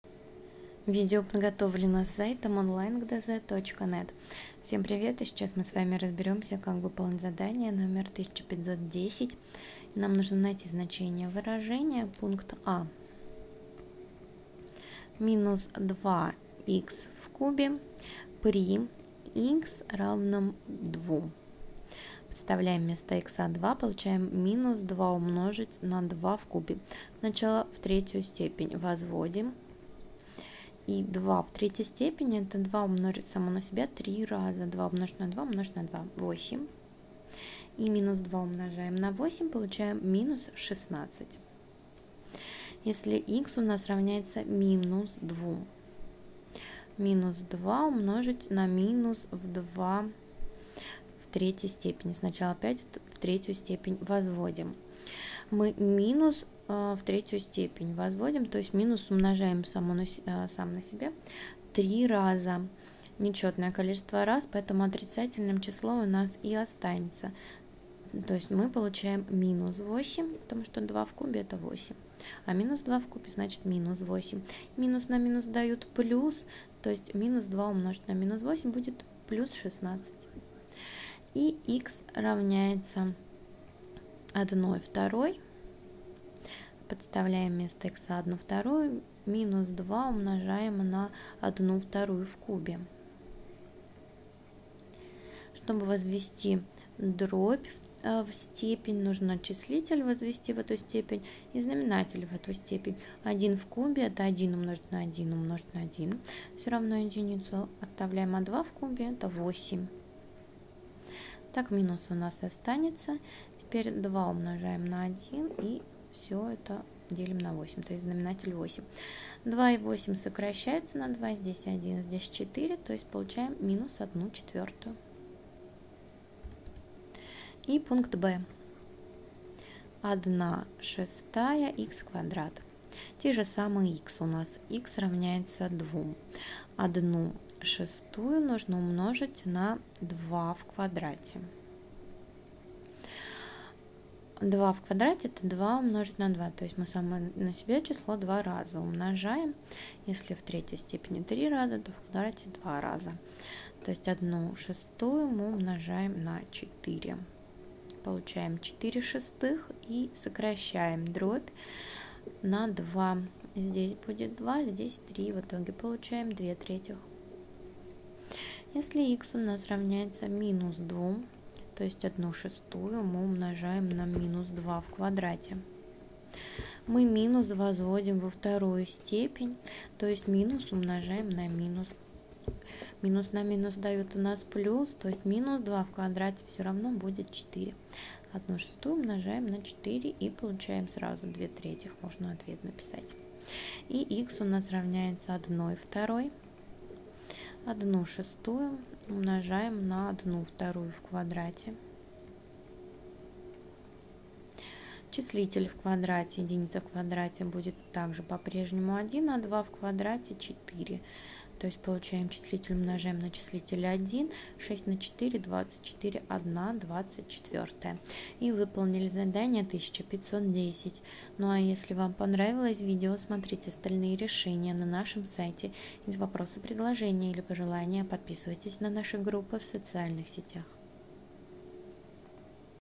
Задание № 1510 - Математика 6 класс (Виленкин, Жохов)
Задание 1510, как и все задания 48 последнего раздела учебника — задание на повторение изученного за год материала. А точнее, вам следует вспомнить то, что вы изучали в 7 параграфе этого учебника (умножение и деление положительных и отрицательных чисел) и то, что вы изучали еще в 5 классе (возведение числа в степень). Если вы хотите без ошибок сделать это задание, посмотрите наше видео.